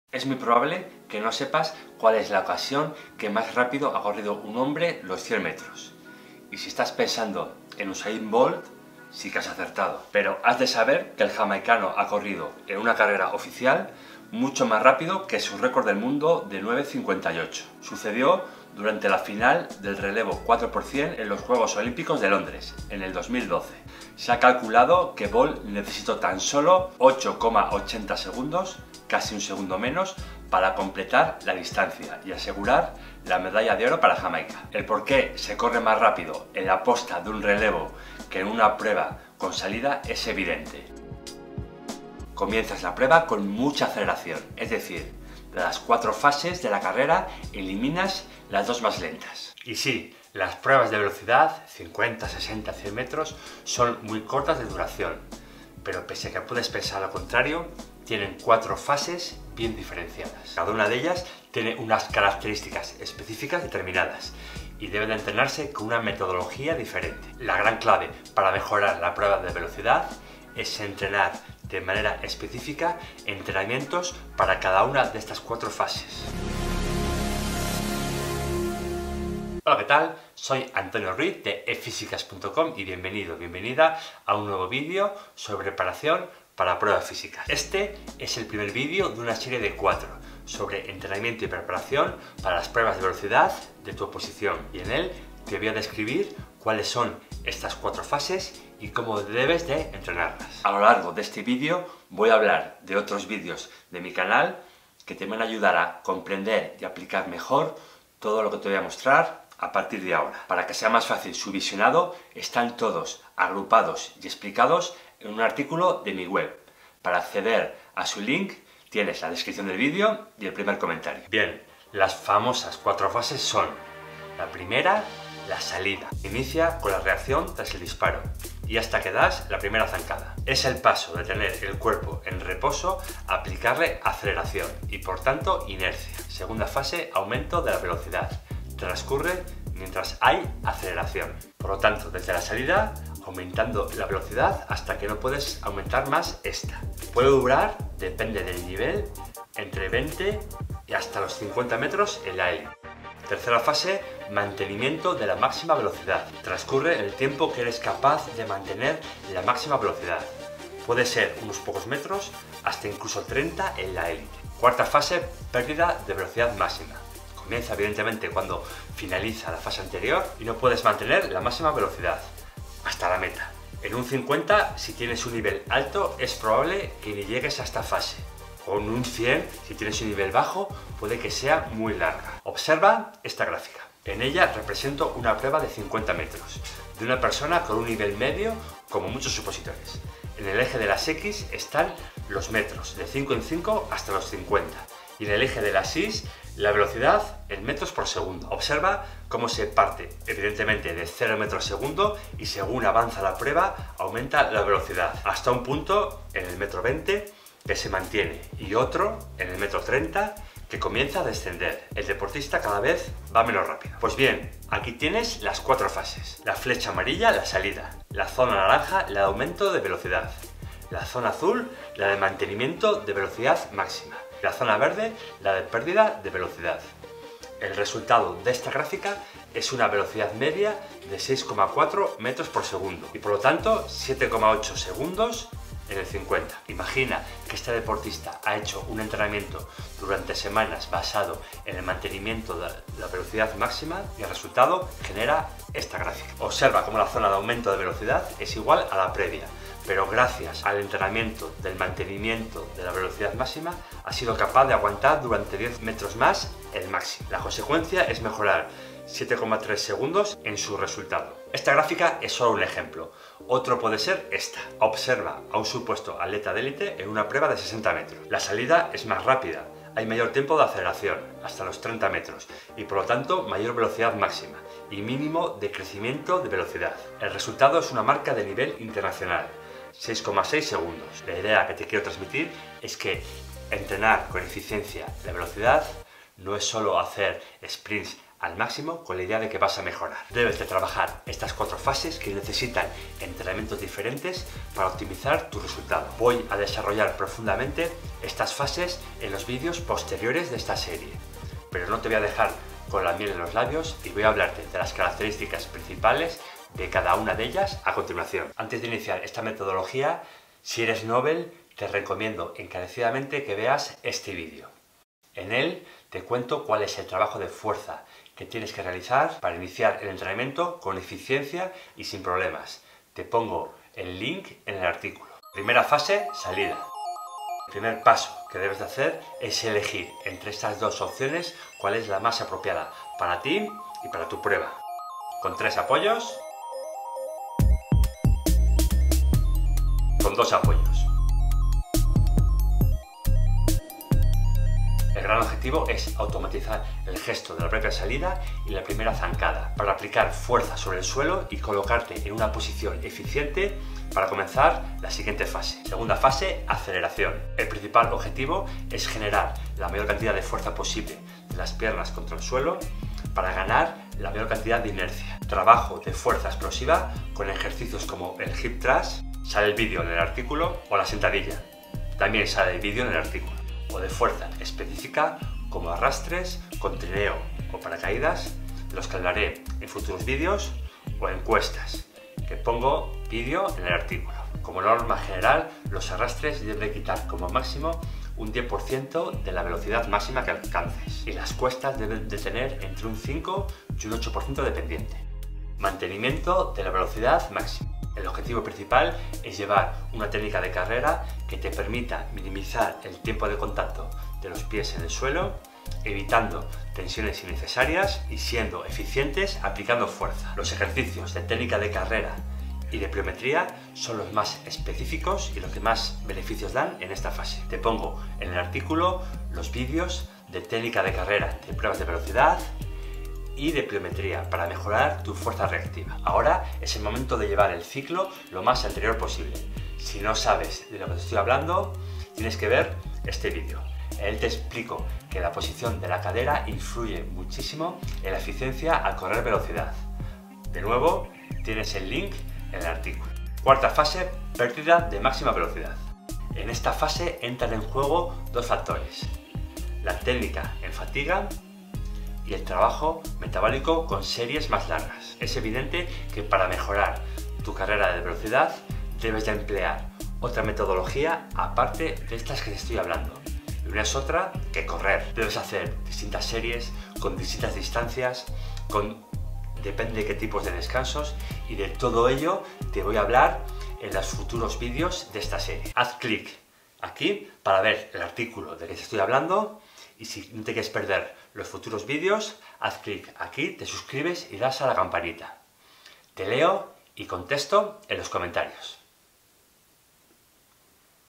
🚀 Como ENTRENAR la carrera de VELOCIDAD (50 - 60 y 100 metros).
Una prueba de velocidad es muy corta pero... ¿Sabias que se divide en cuatro fases y que cada una tiene metodologías diferentes de entrenamiento?

No te pierdas ningún video sobre preparación para Pruebas Físicas.